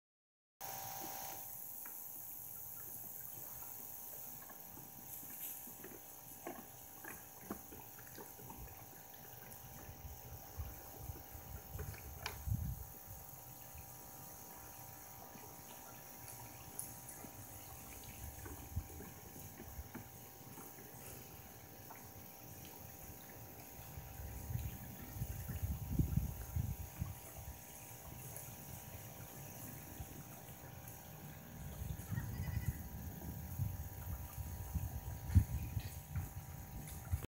4 week old doelings playing
Cookie and Katara playing around the adults after Goat Sunday.